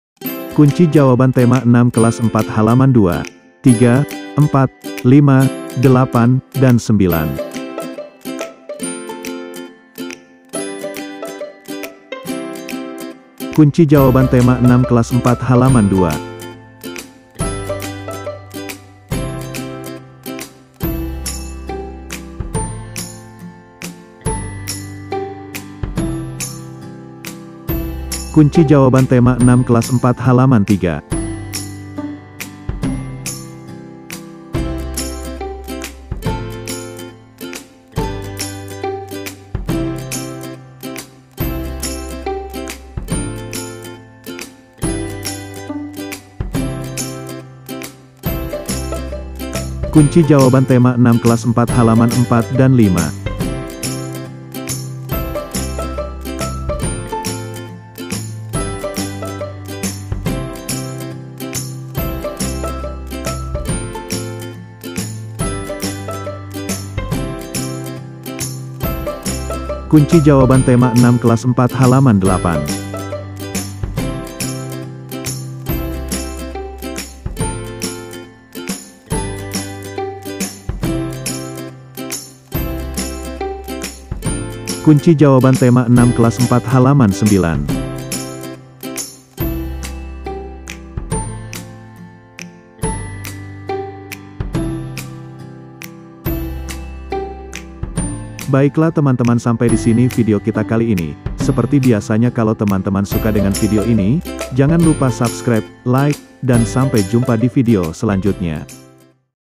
Kunci Jawaban Tema 6 Kelas 4 Halaman 2 3 4 5 8 9 | Subtema 1 Pembelajaran 1
Jawaban Tema 6 Kelas 4 Halaman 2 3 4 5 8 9 | Subtema 1 Pembelajaran 1
Yang mau traktir kopi untuk mendukung channel ini, silahkan klik link dibawah ini, trim’s
00:00 Judul
00:14 Jawaban Tema 6 Kelas 4 Halaman 2
00:28 Jawaban Tema 6 Kelas 4 Halaman 3
00:50 Jawaban Tema 6 Kelas 4 Halaman 4 dan 5
01:09 Jawaban Tema 6 Kelas 4 Halaman 8
01:24 Jawaban Tema 6 Kelas 4 Halaman 9
01:38 Video Selanjutnya Jawaban Tema 6 Kelas 4 Halaman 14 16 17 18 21
Source :
• Buku Guru SD/MI Kelas IV Tema 6 Cita-citaku Buku Tematik Terpadu Kurikulum 2013 Edisi Revisi 2017. Jakarta: Kementerian Pendidikan dan Kebudayaan.
• Buku Siswa SD/MI Kelas IV Tema 6 Cita-citaku Buku Tematik Terpadu Kurikulum 2013 Edisi Revisi 2017. Jakarta: Kementerian Pendidikan dan Kebudayaan.
• Berbagai sumber lainnya